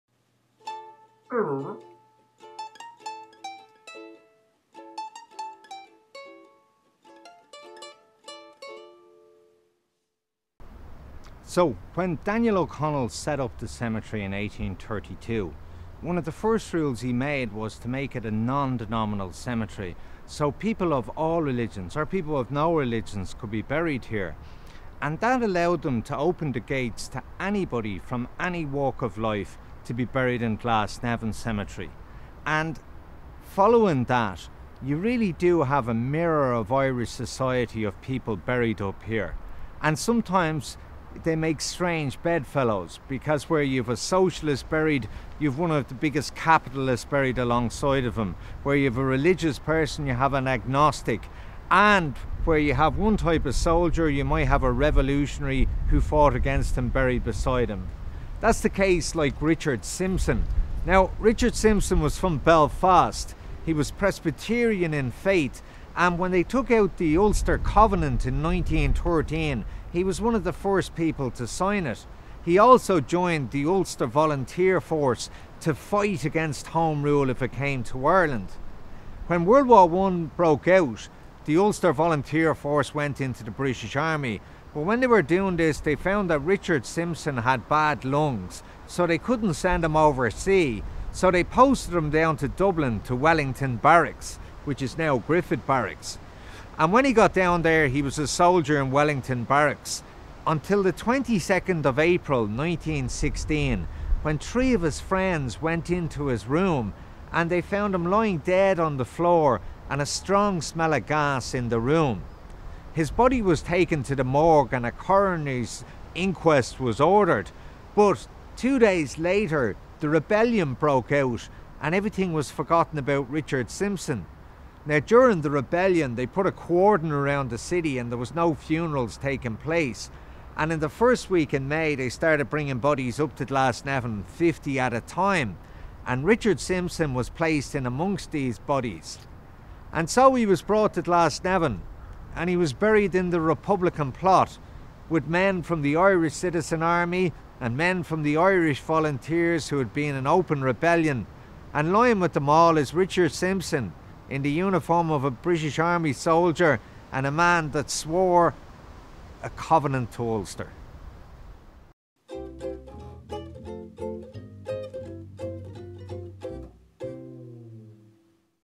Shane - Strange bedfellows
Among the graves in Glasnevin there are plenty of surprising neighbours. Shane tells about one UVF soldier who ended up in the least likely of places.